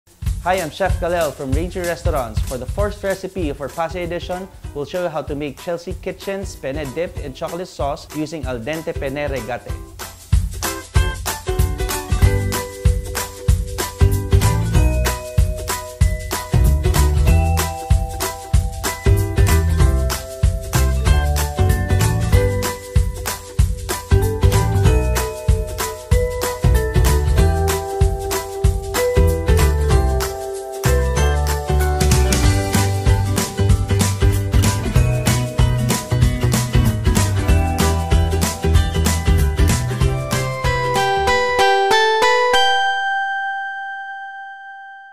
Doña Elena | Chef Kalel's Penne Dipped in Chocolate Sauce
Celebrate your special someone with the perfect pairing. We’re talking chocolate and pasta. Yes, it’s possible! Let Chef Kalel show you how to craft this dish with Doña Elena Penne Rigate that is sure to win hearts.

Get a chance to win special gift bag from us! All you have to do is post a photo or video of your version of this recipe with Doña Elena Al Dente Pasta on the comment box below. We’ll be announcing the winners real soon!

PENNE RIGATE DIPPED IN CHOCOLATE SAUCE
Serves 2

Ingredients
1 cup Belgian chocolate, melted
1/2 cup Penne Rigate
1 tbsp dried apricot, chopped
1 tbsp dried cranberries, chopped
1 tbsp walnuts, chopped

Procedure:
Cook the Penne Rigate until al dente. Set aside until cool.
Line up the cooked Penne Rigate in a foil or mesh then pour in the melted chocolate.
Sprinkle the apricot, cranberries, and walnuts.
Let it cool for 30 minutes, then ready to serve.